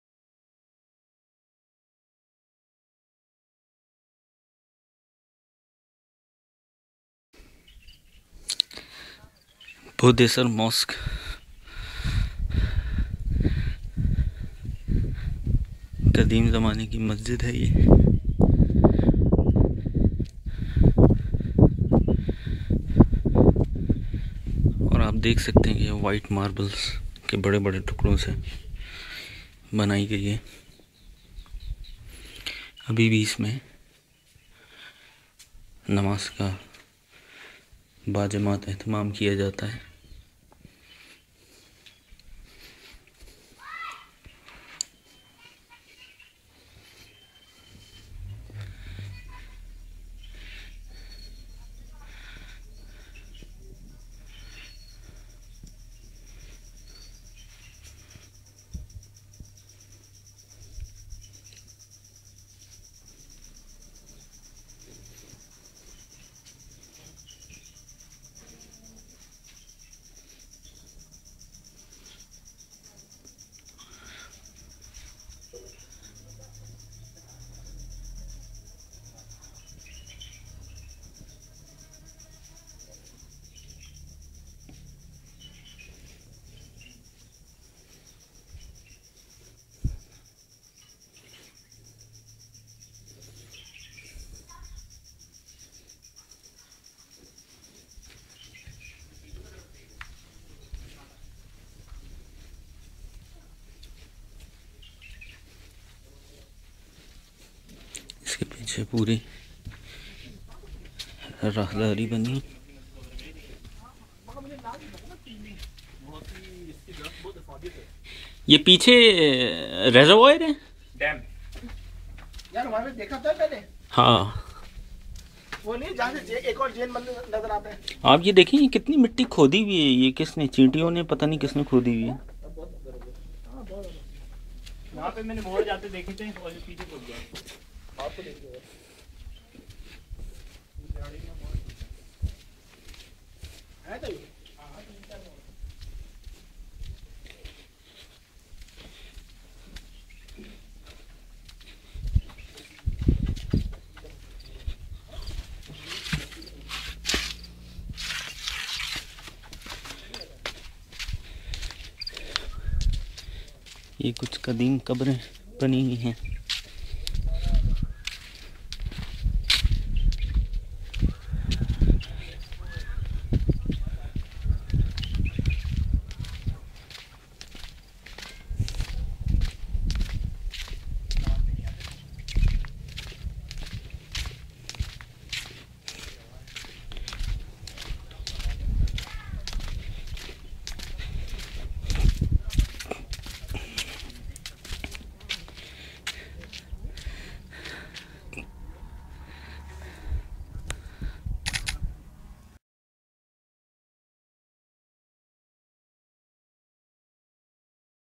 500+ years old Bhodesar Mosque - 2 Days Trip to Nagarparkar, Sindh - Ep 07 of 07
500+ years old Bhodesar Mosque - 2 Days Trip to Nagarparkar, Sindh - Ep 07 of 07